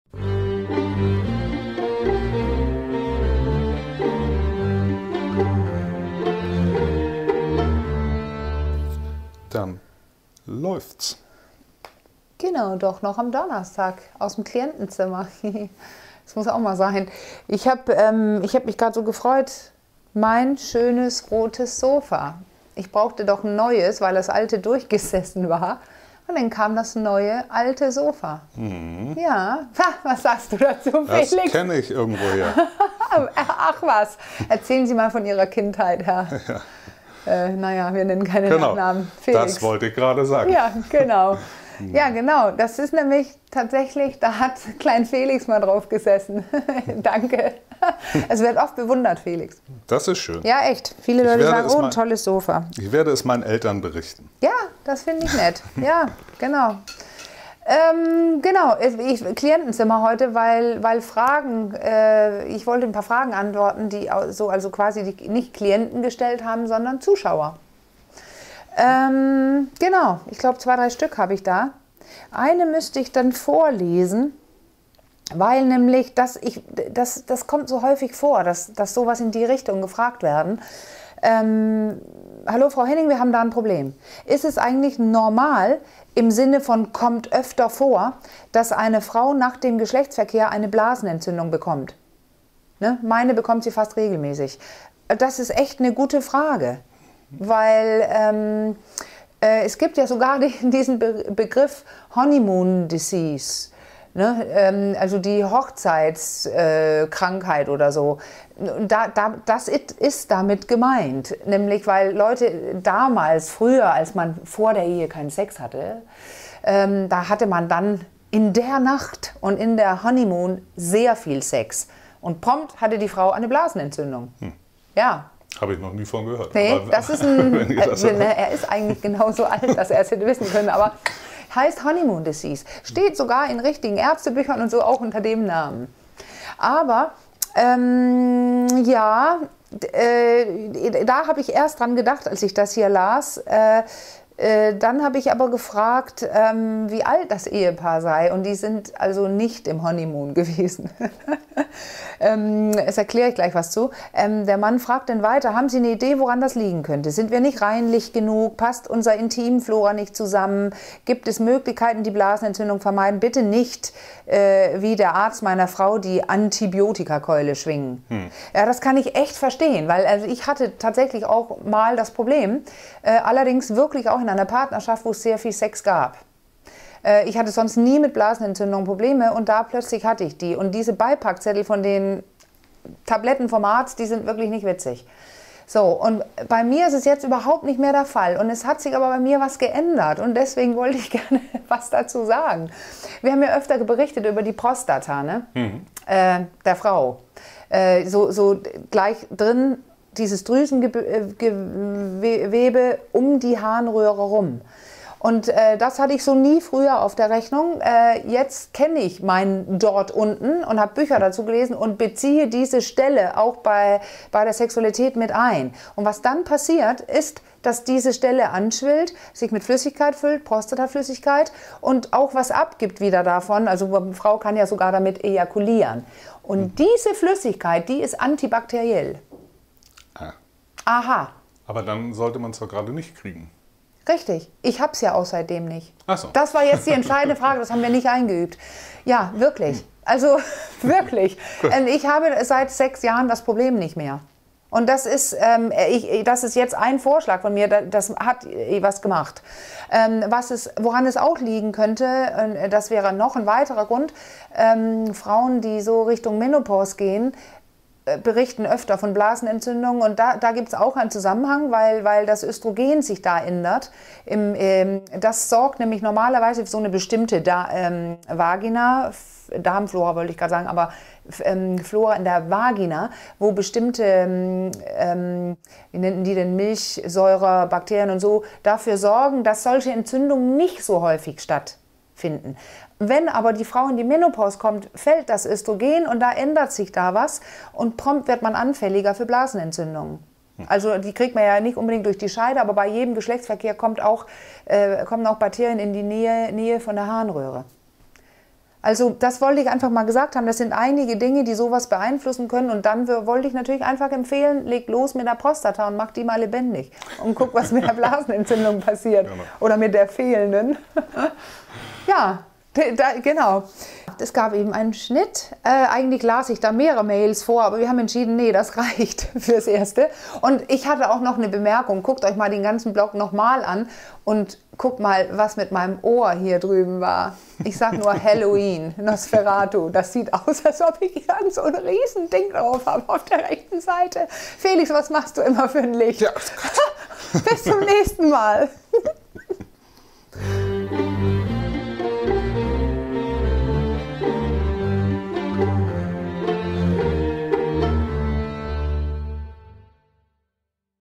Blasenentzündung durch Sex?
Aua! Gibt es einen Zusammenhang zwischen (viel) Sex und dem Auftreten einer Blasenentzündung? Was ist dran an der Honeymoon-Desease und -- viel interessanter -- kann man etwas dagegen tun ohne gleich Antibiotiks schlucken zu müssen? Ja, das geht: Das Stichwort lautet "Prostata-Animation" -- bei der Frau!